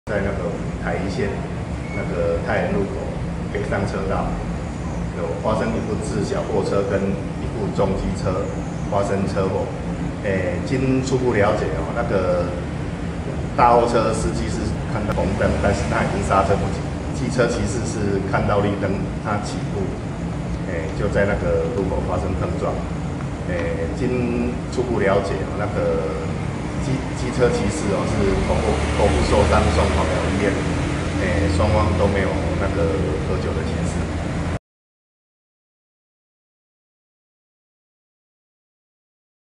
驚悚影片！枋寮省道貨車闖紅燈 撞飛起步女騎士(翻攝畫面)
新聞連結：
省道台1線與枋寮鄉的太源路口是大貨車及遊客南來北往的必經之路，加上車禍飛快，當地民眾橫向出入也相當頻繁，日前就發生大貨車闖紅燈，竟把1位也最快起步的女騎士撞飛，被撞當下影片曝光，驚悚畫面令人心驚肉跳。